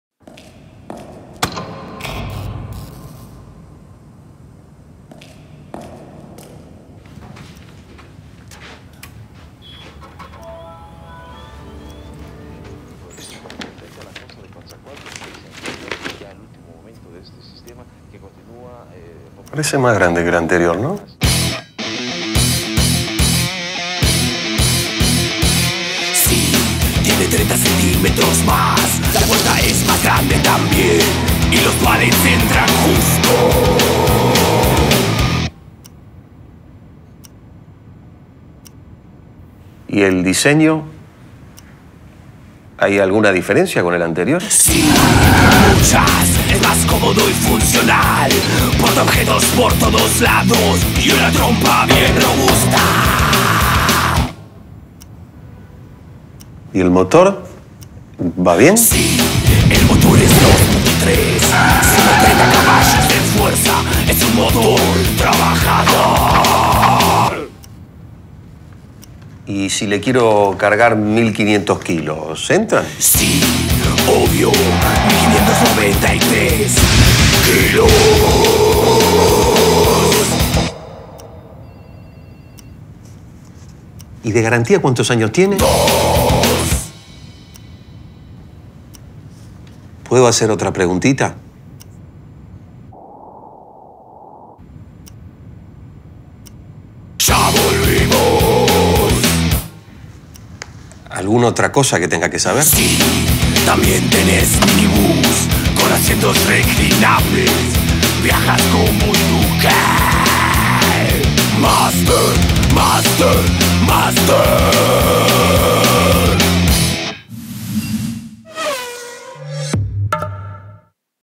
Diego Valdez y Christian Abarca -Publicidad de Renault Master
Publicidad de Renault Master en la cual Participaron  Diego Valdez y Christian Abarca de la banda Helker a mediados del 2013.